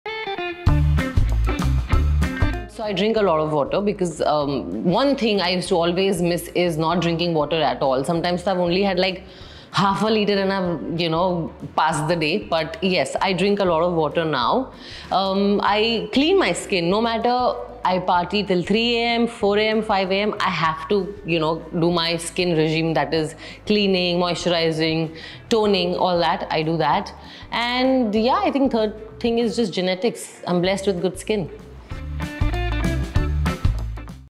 Karishma Tanna spills the beans on her top 3 beauty essentials for glowing skin
Karishma Tanna spills the beans on her top 3 tips for achieving that radiant complexion. From her daily skincare routine to nourishing rituals, she's all about embracing a healthy glow inside and out!